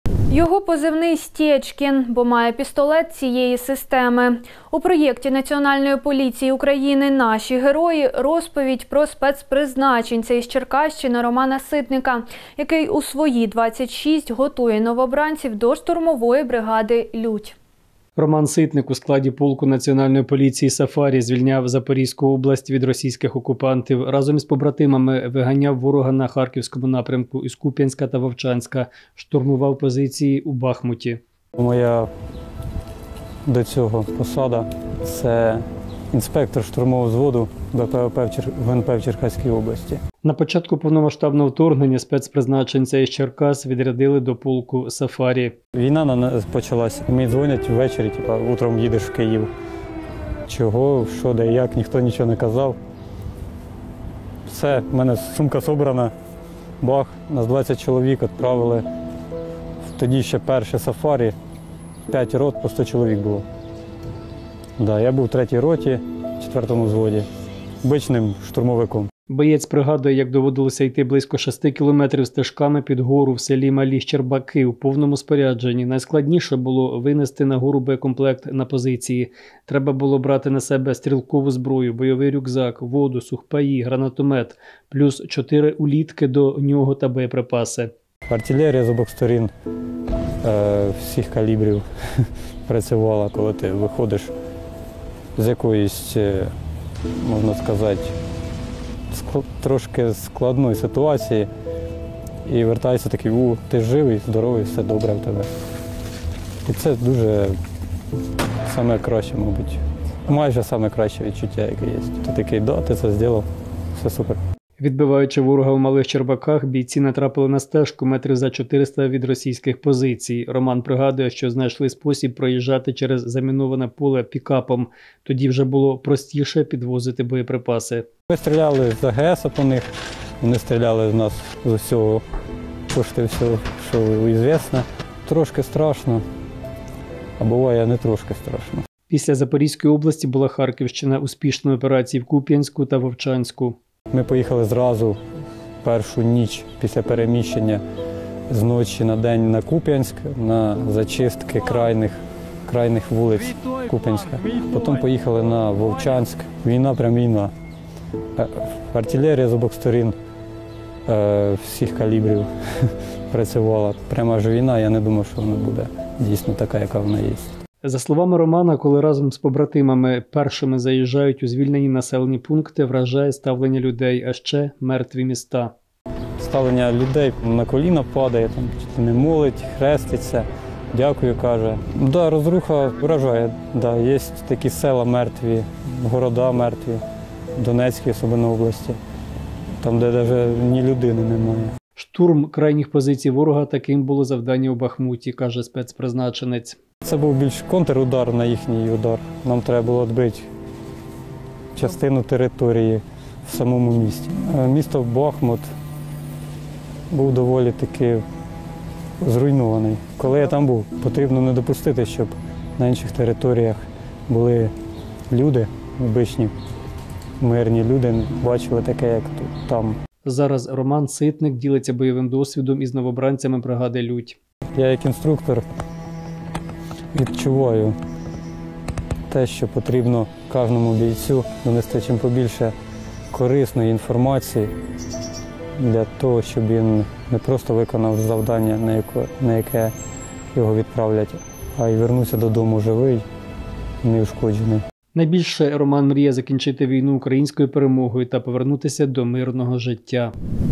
Черкаський «Стєчкін» став наставником для новобранців штурмової бригади «Лють»
Його позивний «Стєчкін», бо має пістолет цієї системи. У проєкті Національної поліції України «Наші герої»  розповідь про спецпризначенця з Черкащини Романа Ситника, який  у свої 26 готує новобранців до штурмової бригади «Лють».